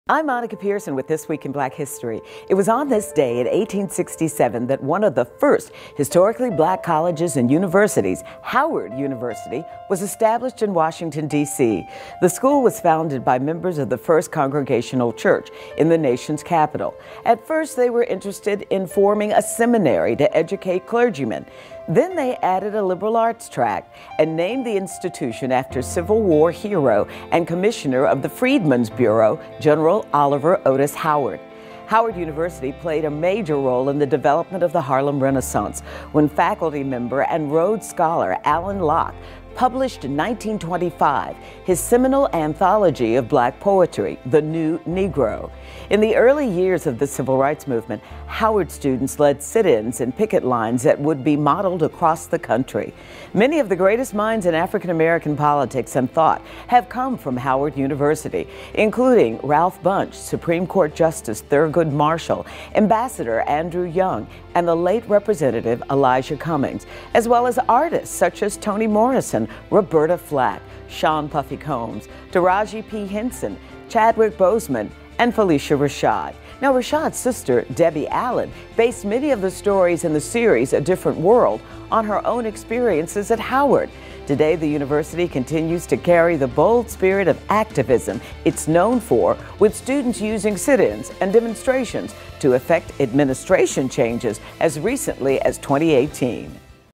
This Week in Black History: March 2 | Howard University
It was on this day in 1867 that Howard University was established! Do we have any Howard alumni here?